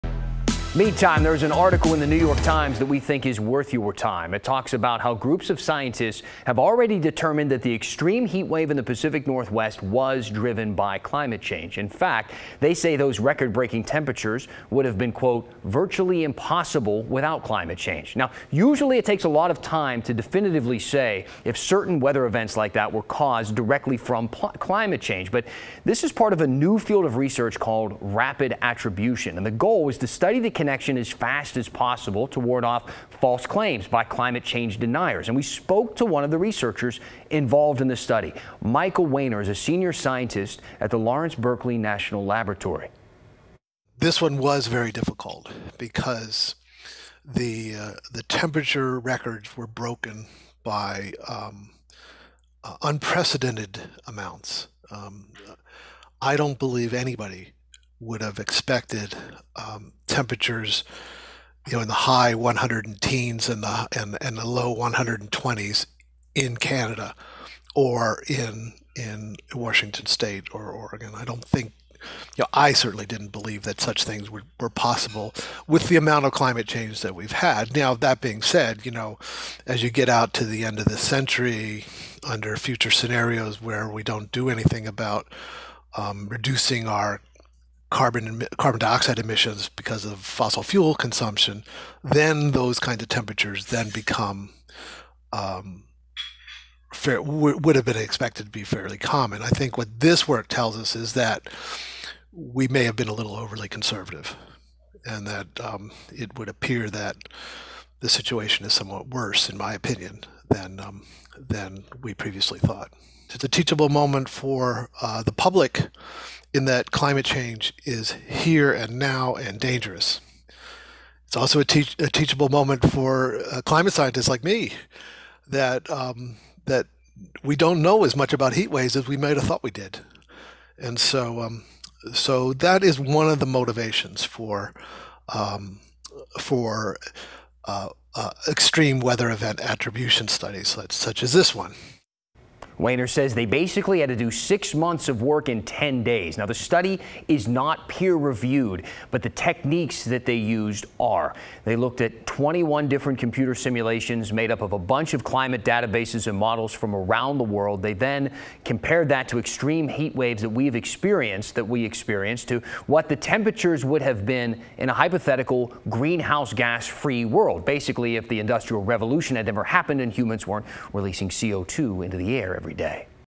NYT: Pacific Northwest heatwave would have been 'virtually impossible without climate change'
Was the deadly Northwest heat wave related to climate change? KGW spoke to a scientist who believes it must have been.